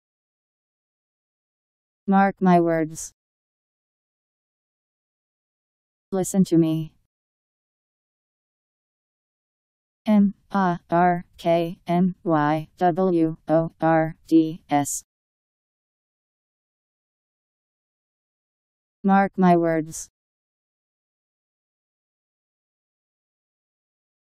Mark my words Meaning
Video shows what mark my words means. Listen to me, ..  Mark my words Meaning. How to pronounce, definition audio dictionary. How to say mark my words. Powered by MaryTTS, Wiktionary